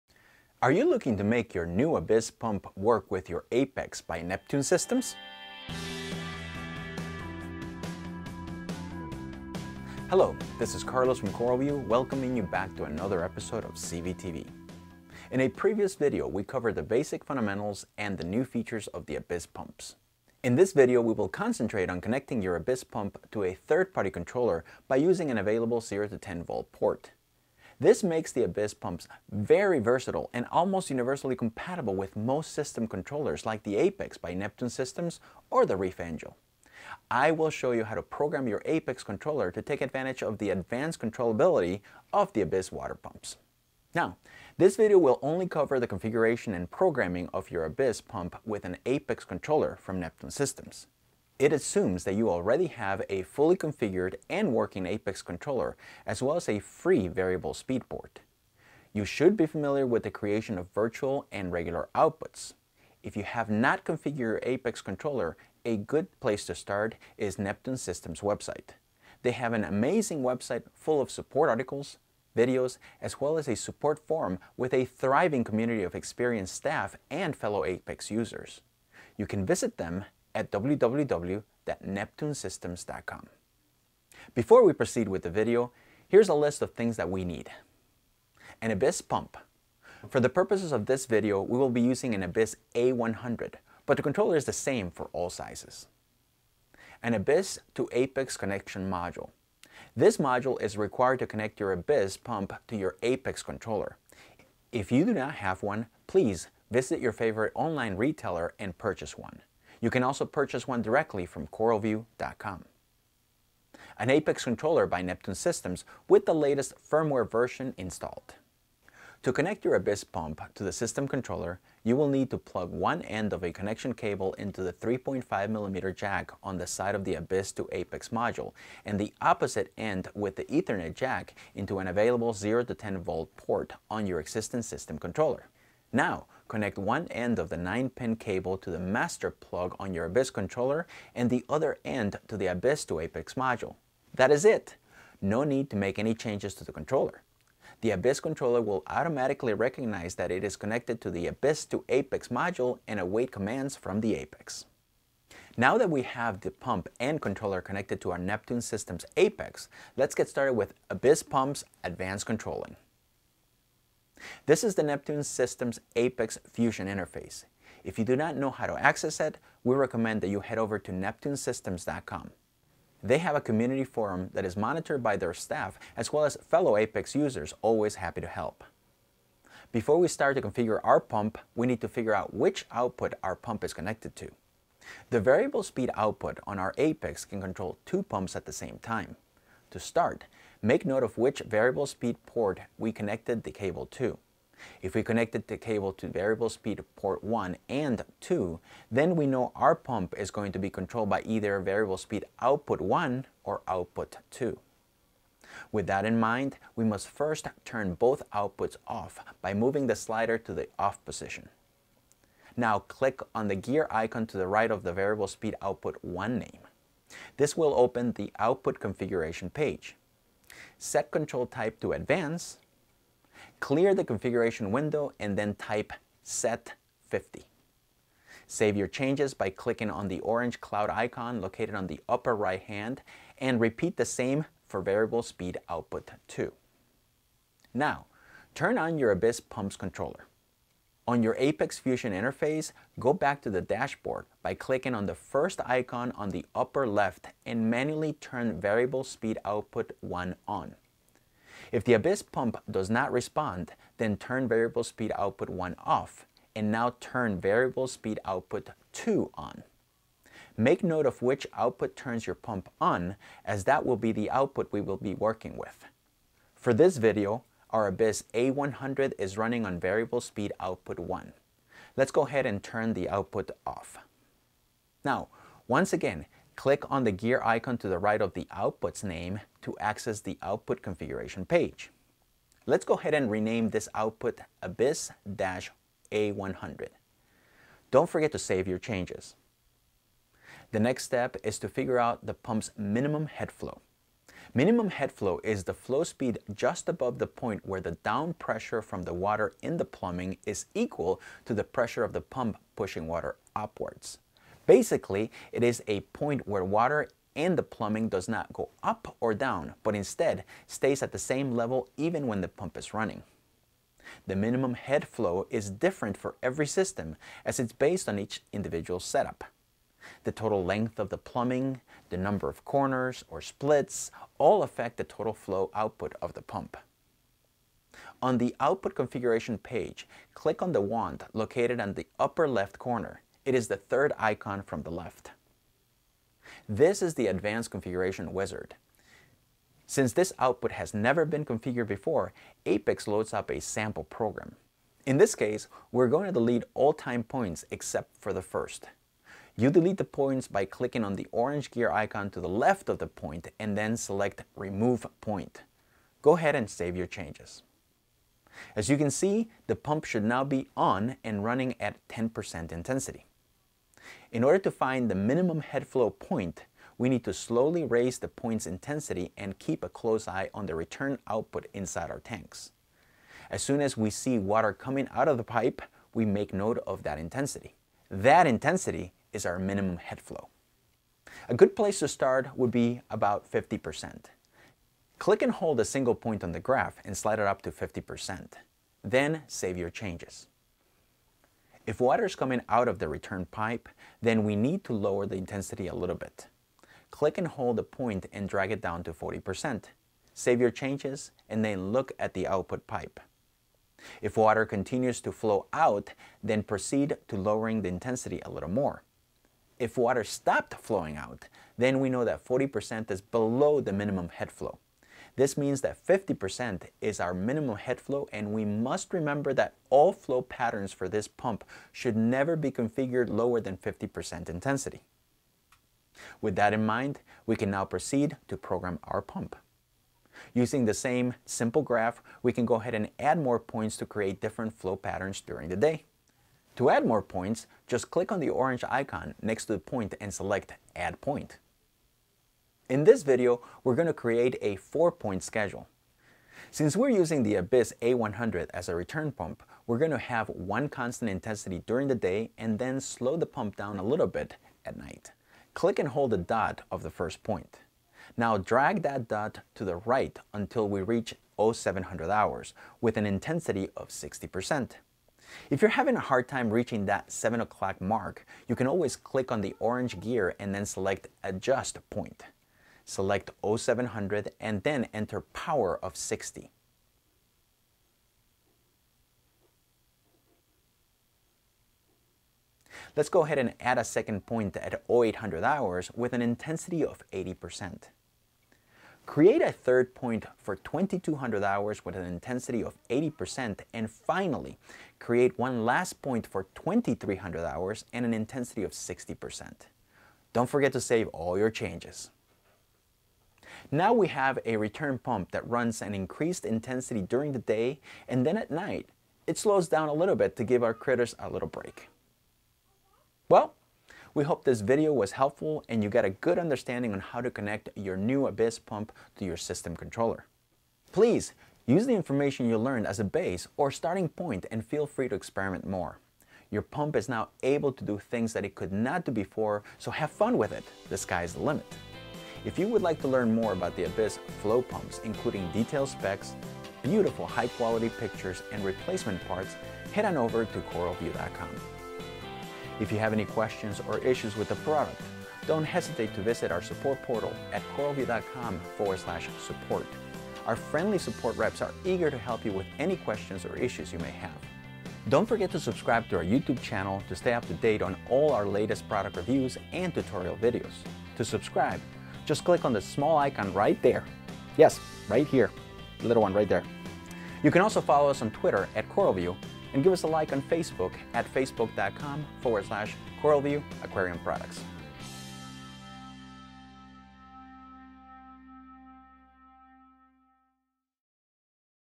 Abyzz Pumps: Advanced Controlling
In this latest episode of [CVtv], Carlos demystifies the task of connecting and programming an Abyzz pump to an APEX controller by Neptune Systems.  See how easy it is to create a schedule that features strong flow during the day but slows down at night.  This is a must watch video for any serious hobbyist.

For more information on the Abyzz to APEX Module, please